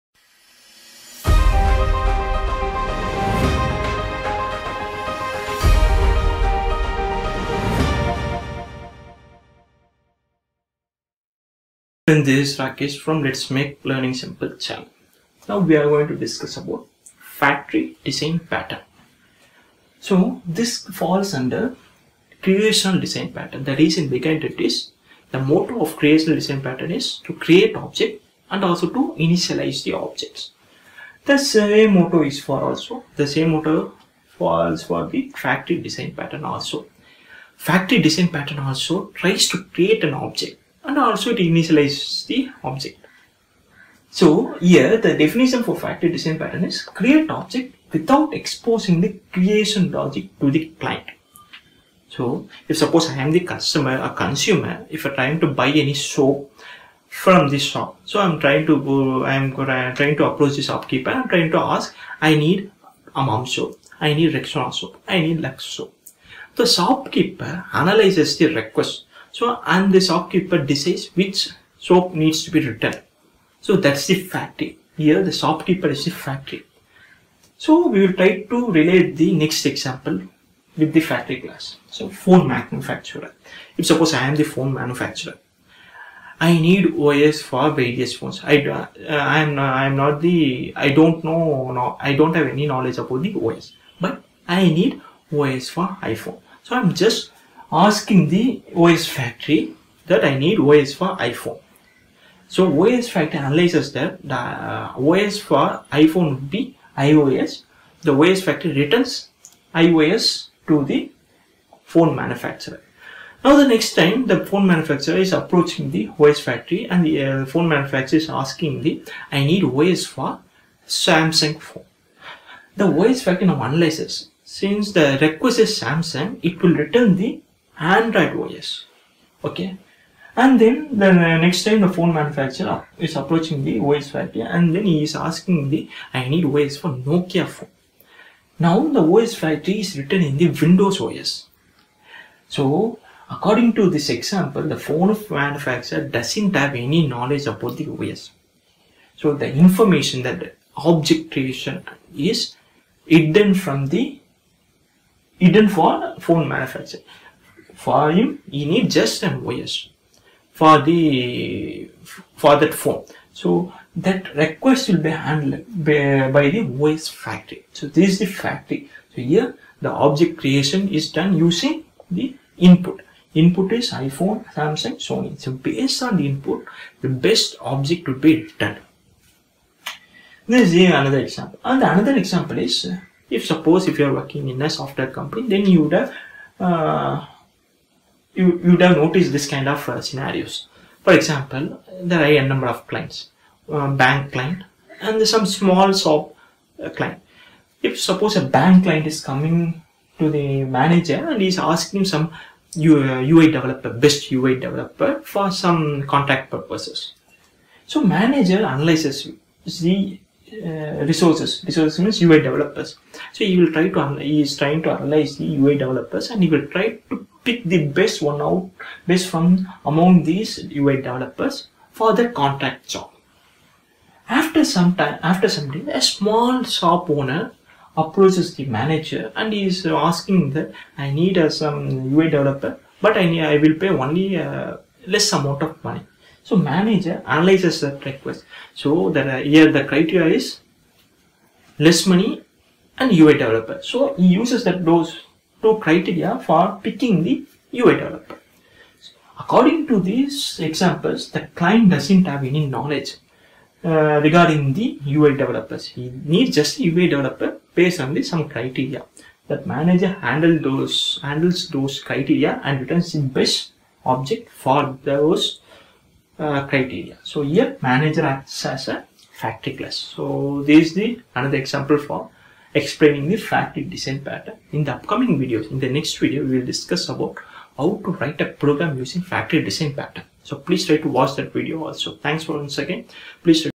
What is Factory Design Pattern? | Factory Design Pattern Real-time Examples
Factory Design Pattern:
→ It falls under Creational Design Pattern.
→ Factory pattern says that object creation will be handled by another class using the input type.
→ In Factory pattern, we create an object by hiding the creation logic.

Advantages:
→ It allows loose-coupling.
→ The end user needn't worry about the technical details about object creation.
→ Factory pattern is widely used in most of the programming languages like Java, C++, and etc.

When to use the Factory Design Pattern?
→ The factory design pattern is used when we have a superclass with multiple sub-classes and based on the input, we need to return one of the sub-class.
→ Example: If a Restaurant serves Veg food, Non-Veg food, and Italian food.
→ Treat VegFood, NonVegFood, and ItalianFood as three classes whose superclass is Food.
→ If a customer asks "Veg" then Factory method would return the "VegFood" class.

Udemy courses link with huge discount:
Actual price is $20. Discounted price is $13 for the first 1000 students.